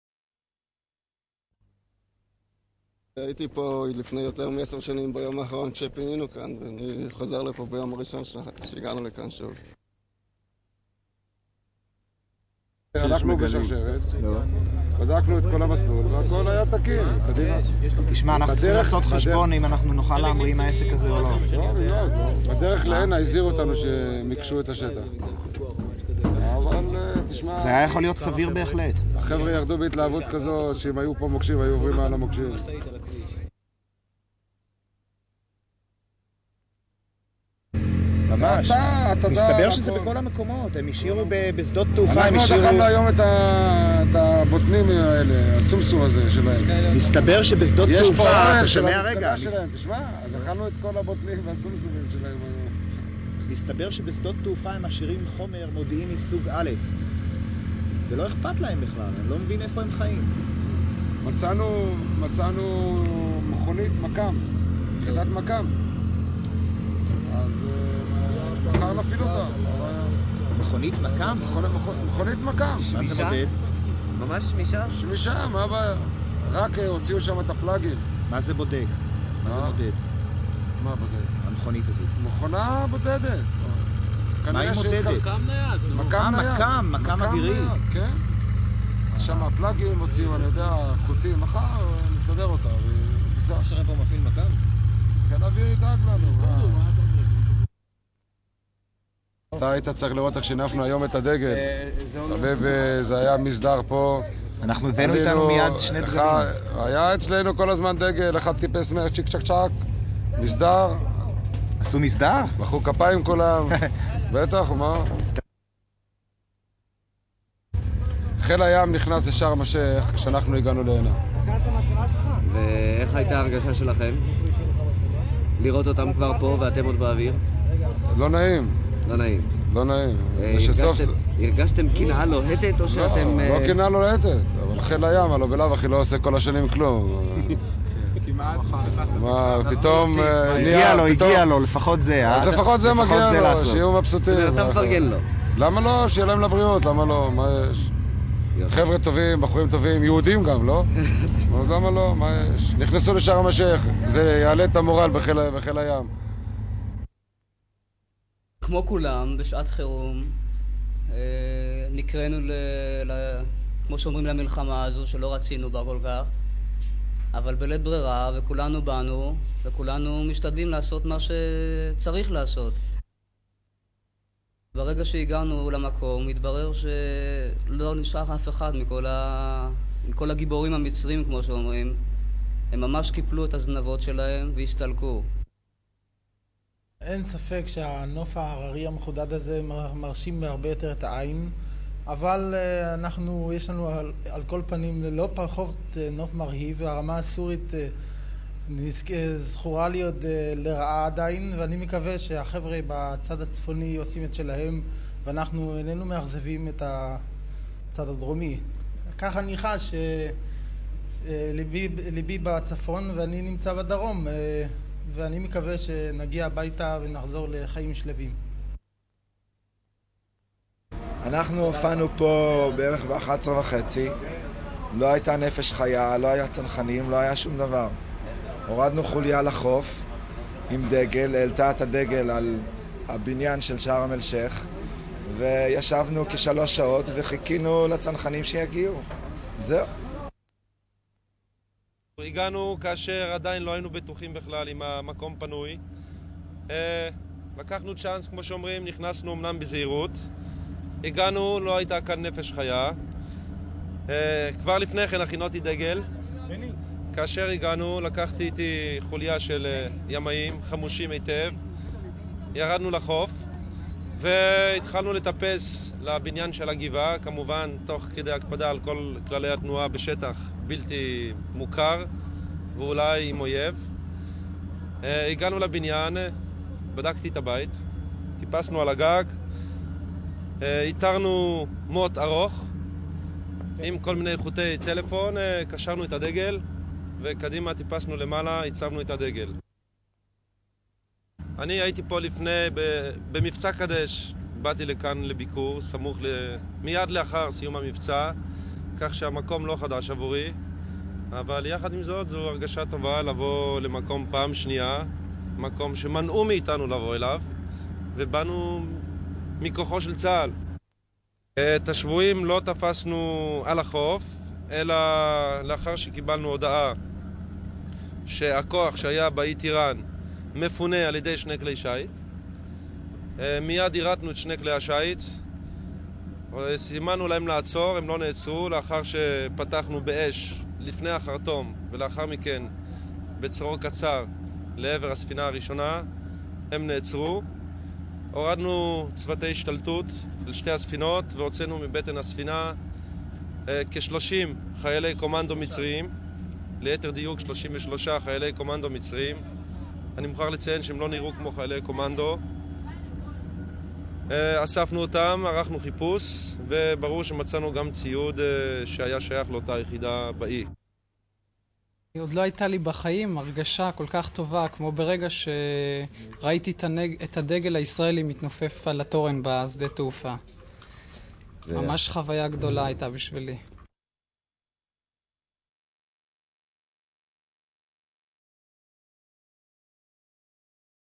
מלחמת ששת הימים 3121-1
סיפורי הנחיתה בשארם א-שייך של חייל הים ומילואי צנחנים  יש גם קטעים מצונזרים של טייסים   .